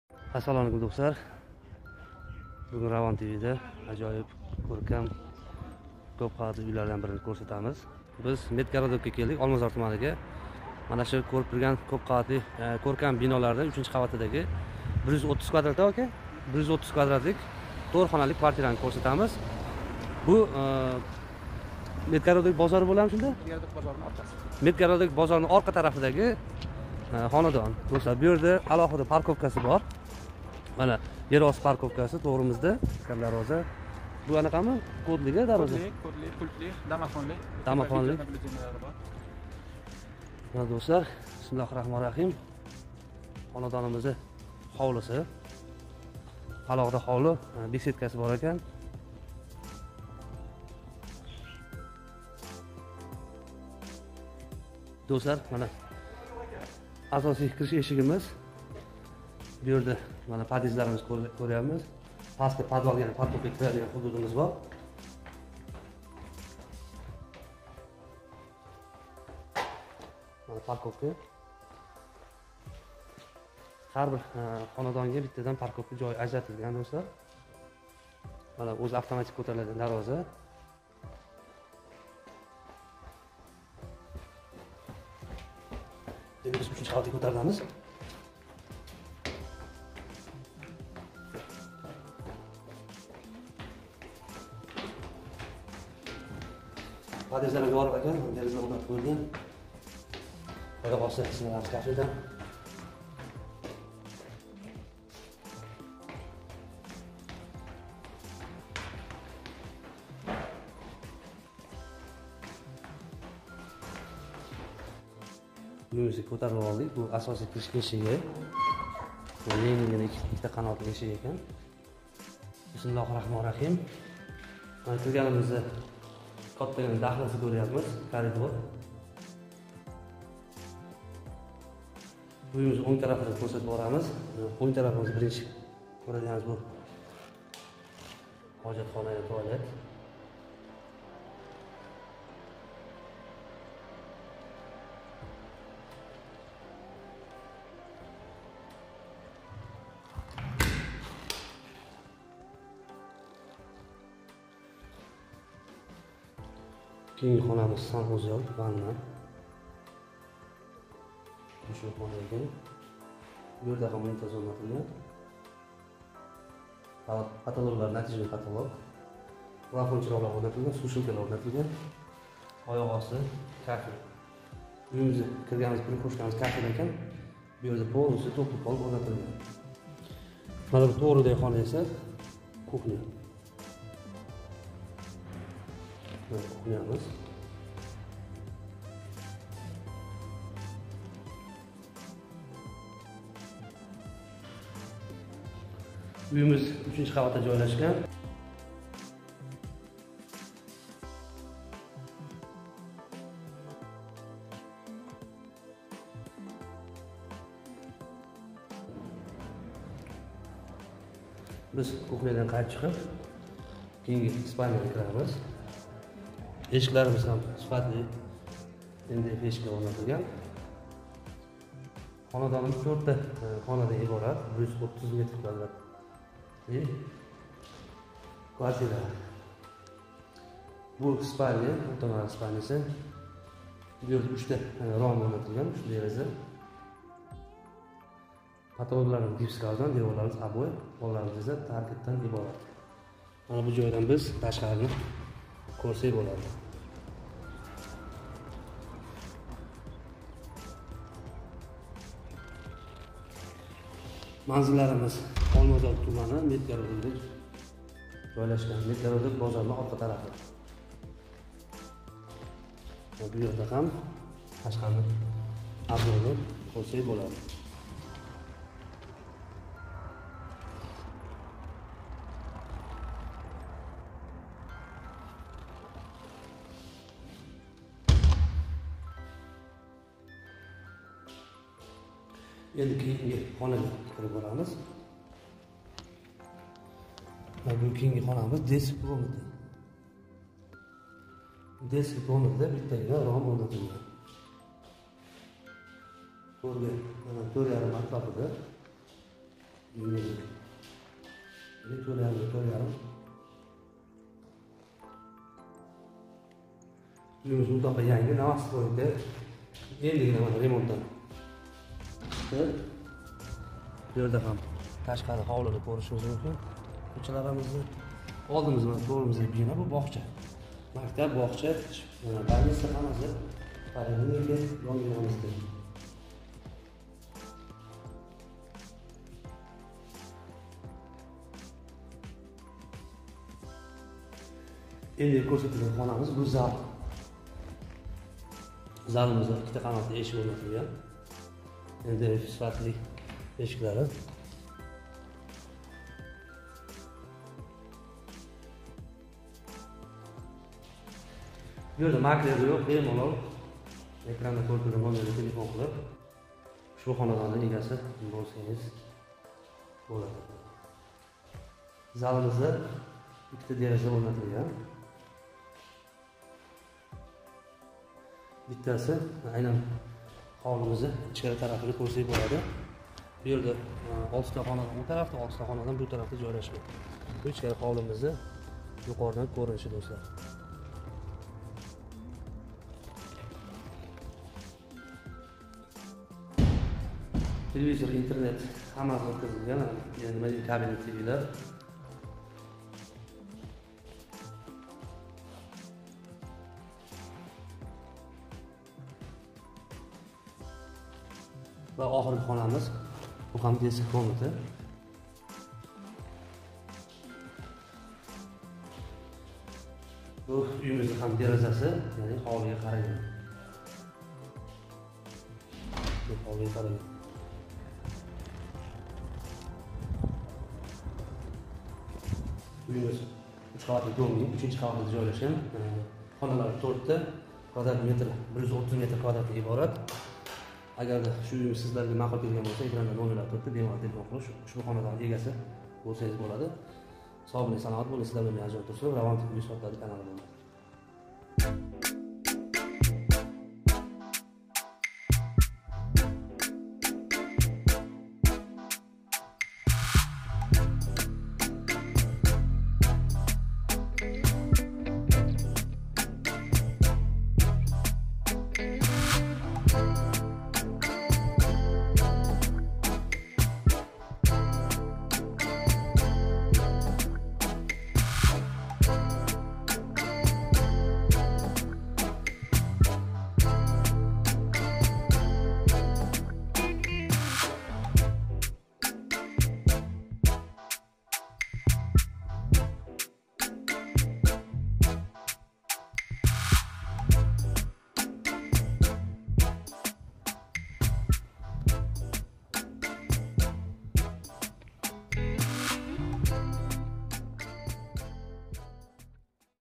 Tashkent Olmazor Medgorodok Novostroyka 4 xona kvartira sotiladi narxi 100000$
Олмазор тумани, Шифокорлар шахарчаси, новостройка 2015 йил битган. 3-этаж. (келажакда 4-этажга мансарда килиш мумкин. Колган кушнилар узлигиси мансардани куриб олишган). Умумий майдон 132 м2.
Евро ремонт. 1 хафта булди ремонт тугатилганига.
1 та зал, 2 та детский, 1 та катта спальни, ошхона ва 2 та сан.узел.
1 та автомобиль учун подвал кисмида парковка ажратилган